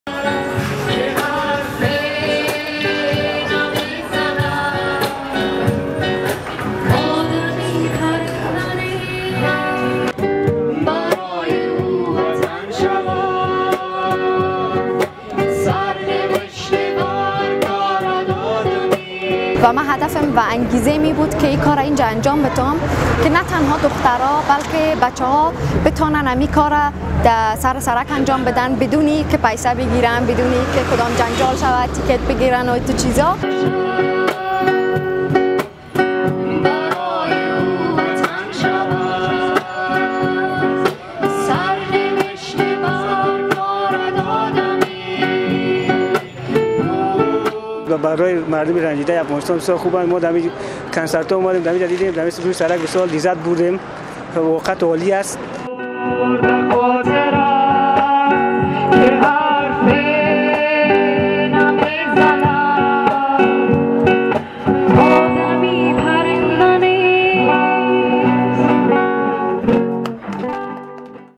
Afghan girl held her first street concert
اولین کنسرت خیابانی یک دختر در کابل
این کنسرت با شعار «نوروز، جنش صلح‌آور» در کارته سه کابل برگزار شد